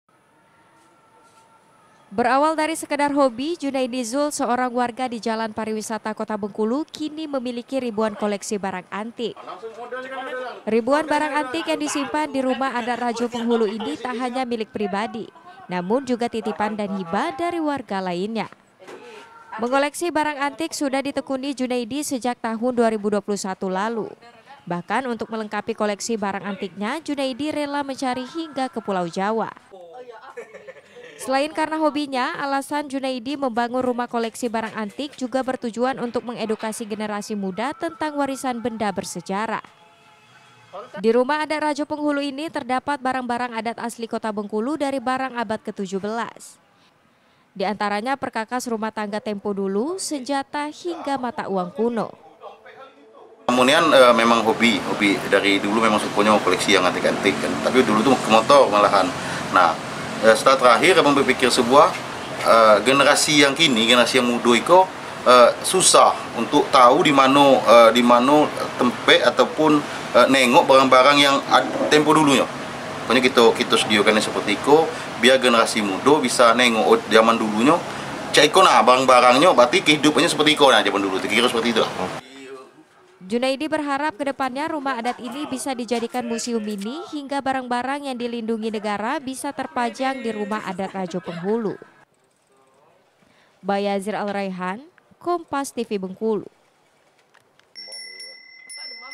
Hobi Koleksi Ribuan Barang Antik untuk Bahan Edukasi
BENGKULU, KOMPAS.TV – Berawal dari sekedar hobi, Junaidi Zul seorang warga di jalan Pariwisata Kota Bengkulu kini memiliki ribuan koleksi barang antic.

Ribuan barang antik yang disimpan di rumah adat rajo penghulu ini tak hanya milik pribadi, namun juga titipan dan hibah dari warga lainnya.

Mengoleksi barang antik sudah ditekuni Junaidi sejak tahun 2021 lalu. Bahkan untuk melengkapi koleksi barang antiknya, Junaidi rela mencari hingga ke pulau jawa.

Selain karena hobinya, alasan Junaidi membangun rumah koleksi barang antik juga bertujuan untuk mengedukasi generasi muda tentang warisan benda bersejarah.

Di rumah adat rajo penghulu ini, terdapat barang-barang adat asli Kota Bengkulu dari barang abad ke 17, diantaranya perkakas rumah tangga tempo dulu, senjata, hingga mata uang kuno.

Junaidi berharap kedepannya rumah adat ini bisa dijadikan museum mini sehingga barang-barang yang dilindungi negara bisa terpajang di rumah adat rajo penghulu.